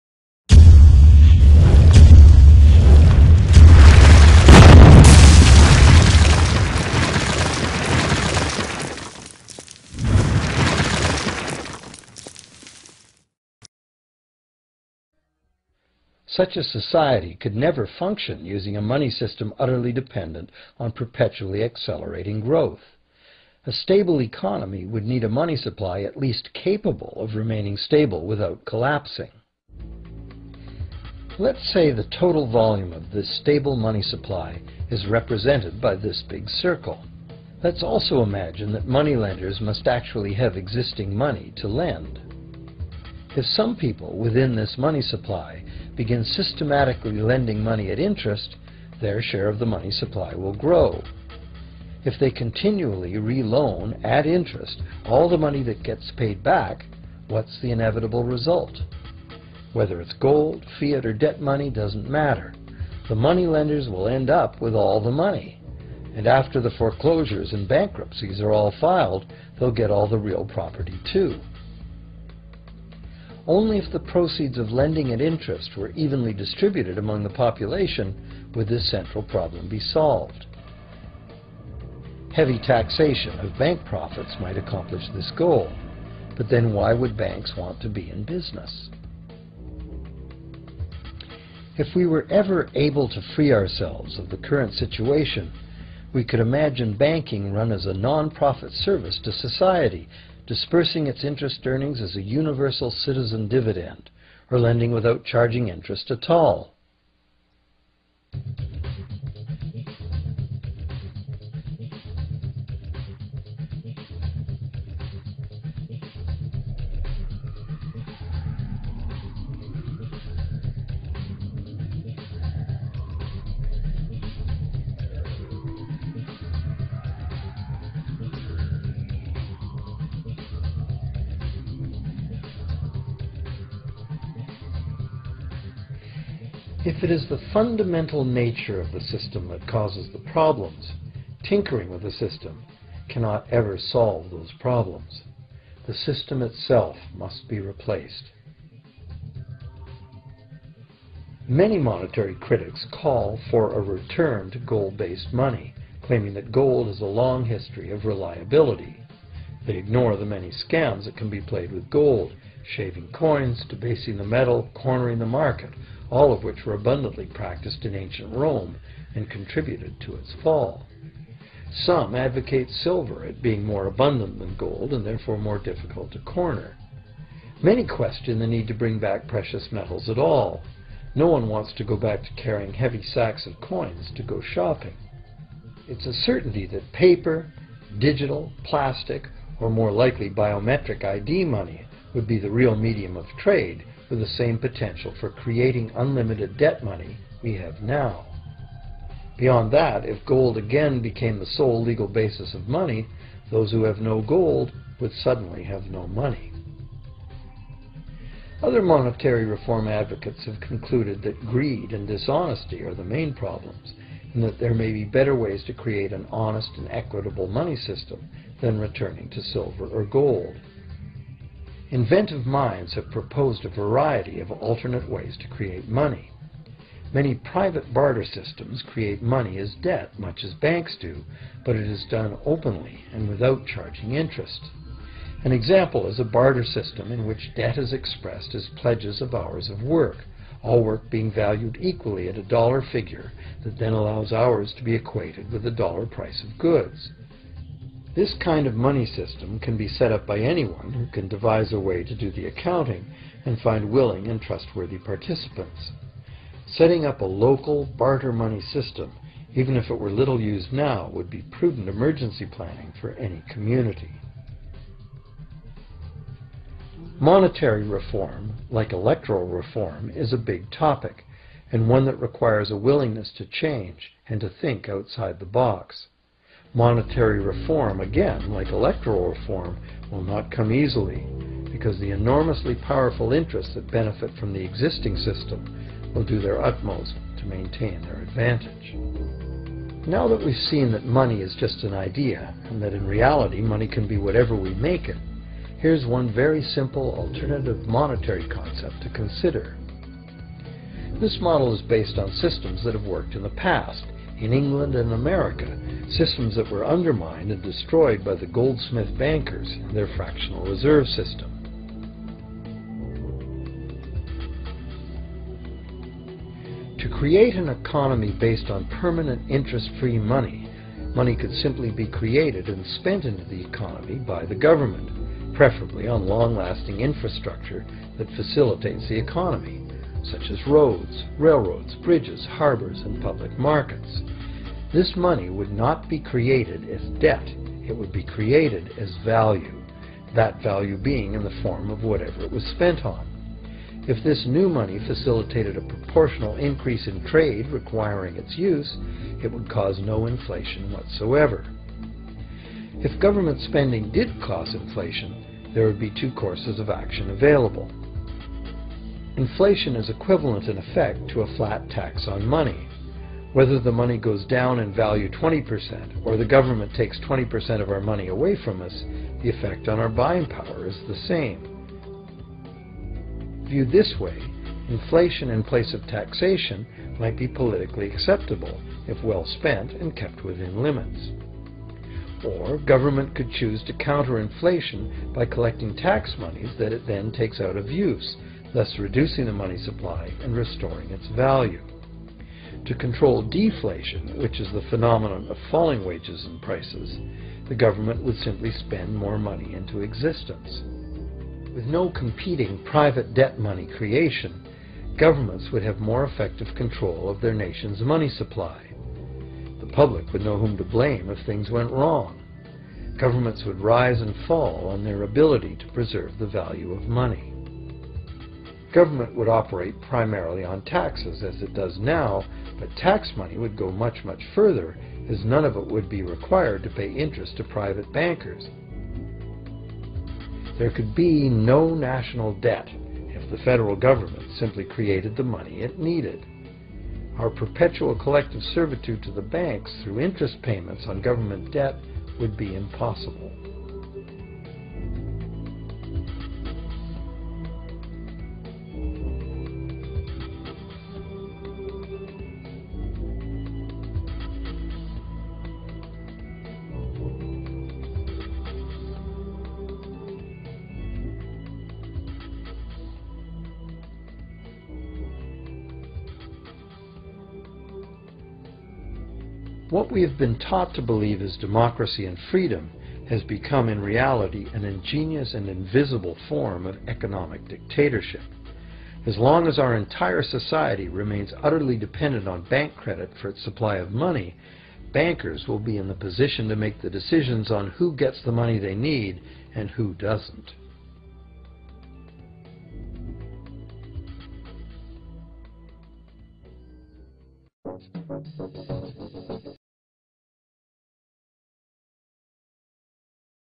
Money as Debt is mathematically IMPOSSIBLE!
Dr. Ron Paul supports money as debt. Bill Still supports public money just as Jefferson, Franklin, and Lincoln.